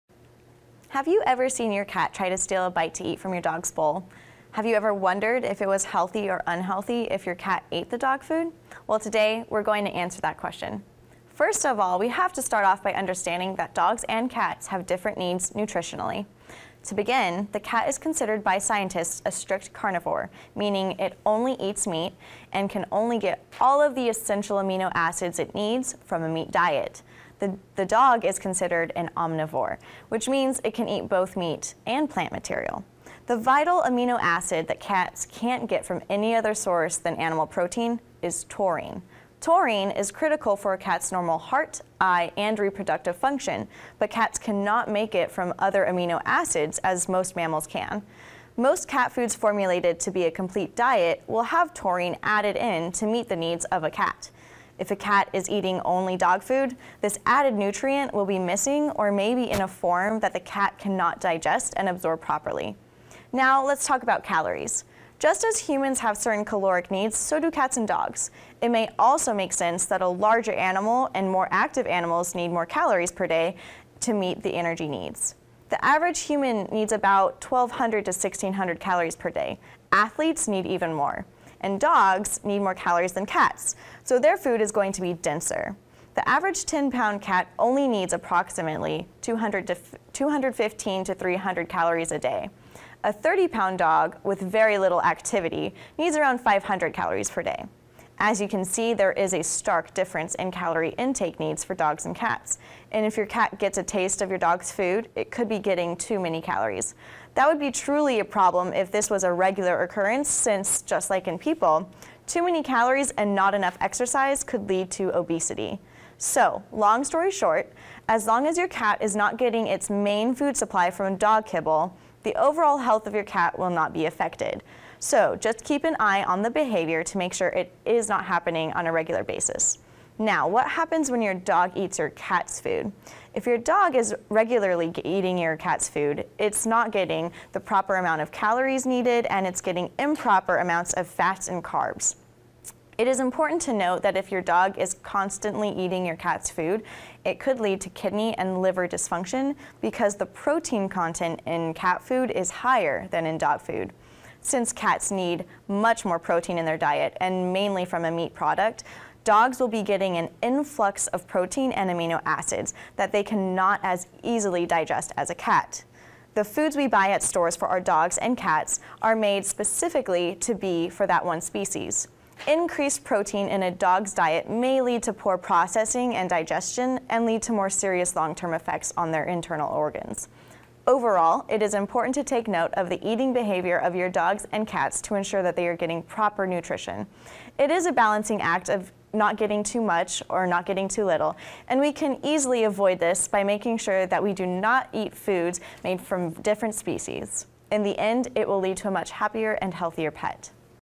Why Can't Cats Eat Dog Food?
Veterinary student, Melodie Raese,  from Texas A&M College of Veterinary Medicine describes the differences in cat and dog nutritional needs. She then explains why cats need their own special diets.